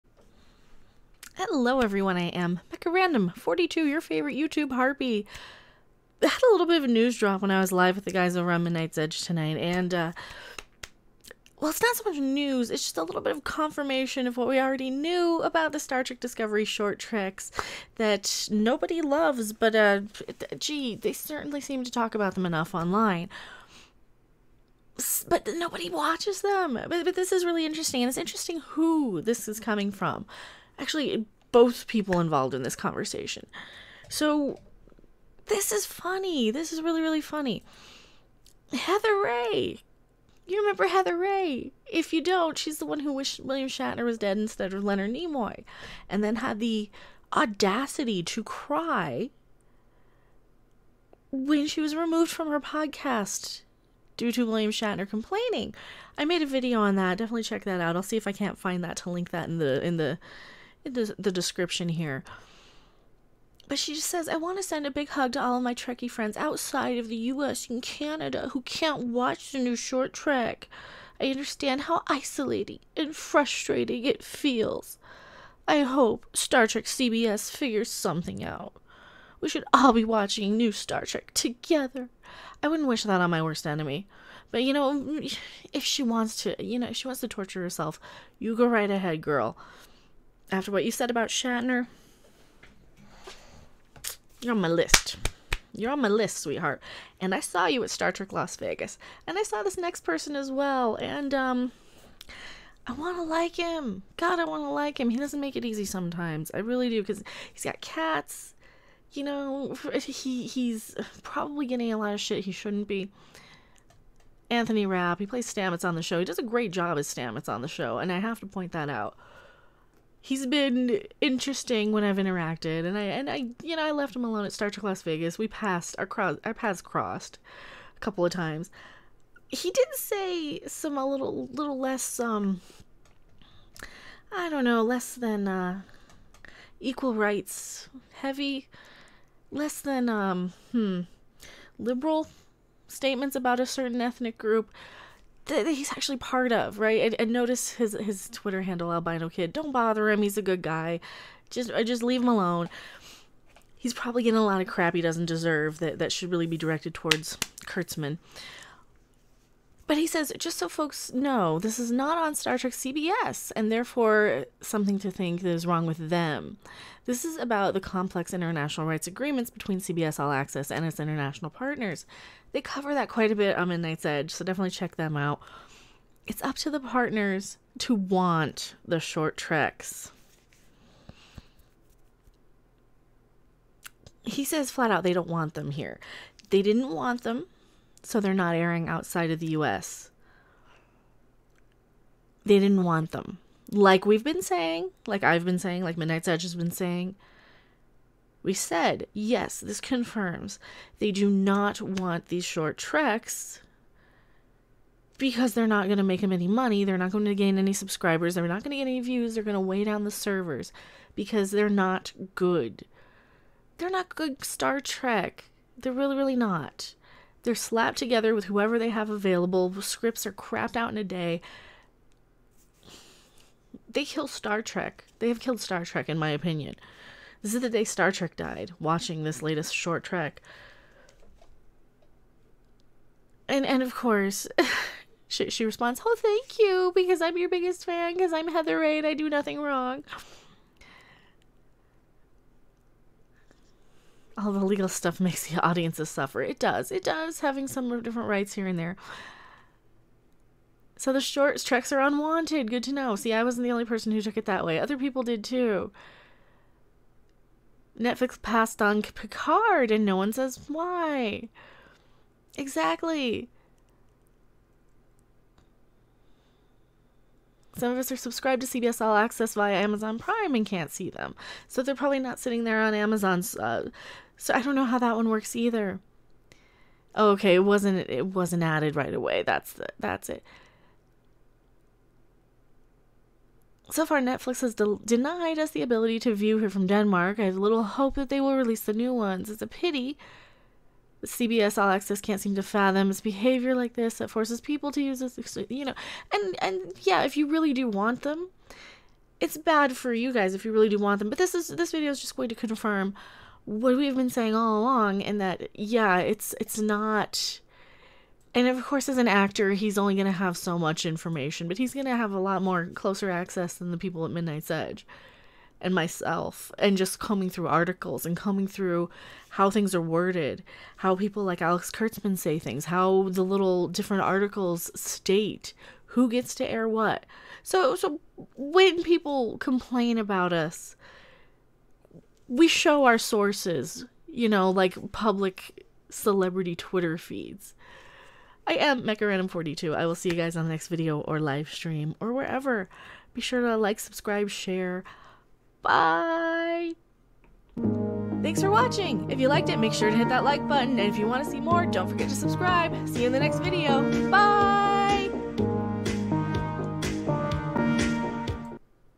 Streaming Services Don't Want Short Treks - Neither Do WE!
Mecha gives further evidence that streaming services passed on Star Trek Discovery supplimental mini episodes Short Treks.

Mecha's Amazon wishlist